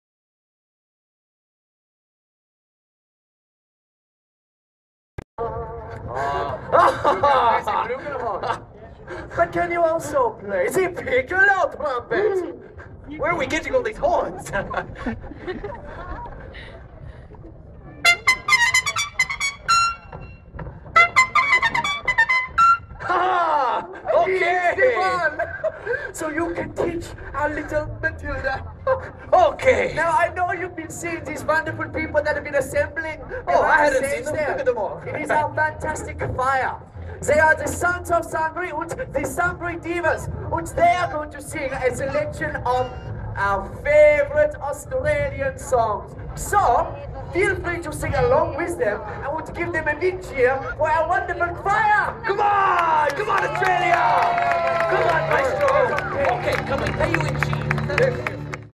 2011 Australia Day Celebrations @ Fed Squre : We've found a horn player for Matilda
2011 Australia Day Celebration at Federation Square Melbourne.

Segment from the performance named 'We've found a horn teacher for Matilda'